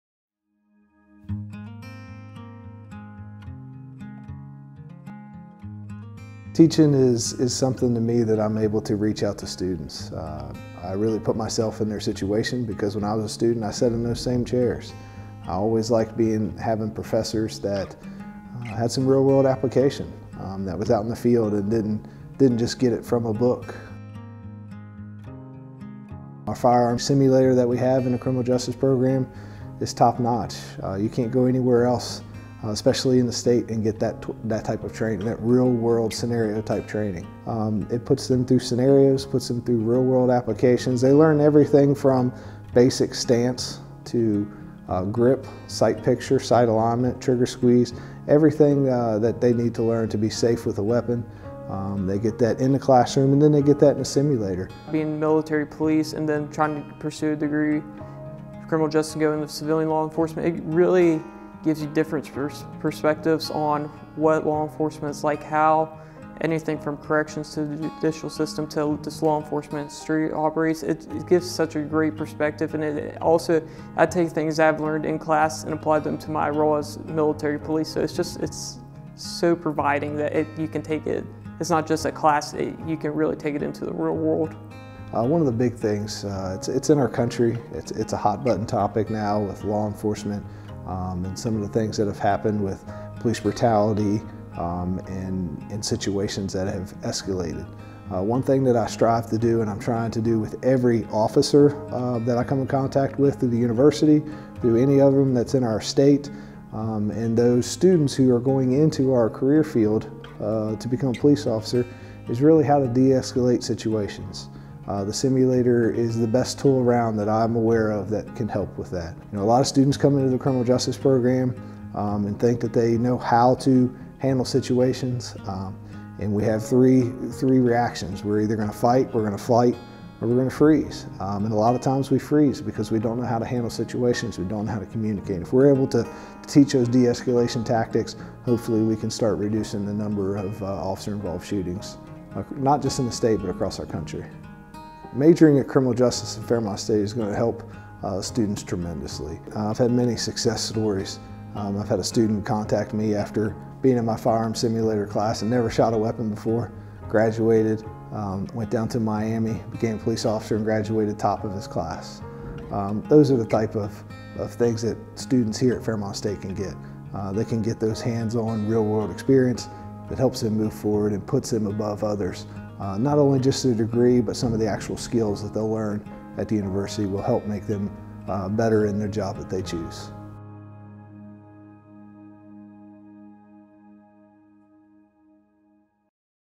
Fairmont State University – Falcon Spotlight: Criminal Justice Virtual Firearms Simulator
At FSU, Criminal Justice majors receive hands-on experience with our state-of-the-art firearms simulation lab. Hear how this makes a difference not only in the classroom but in the real world as well.

Learn more about majoring in Criminal Justice at Fairmont State University​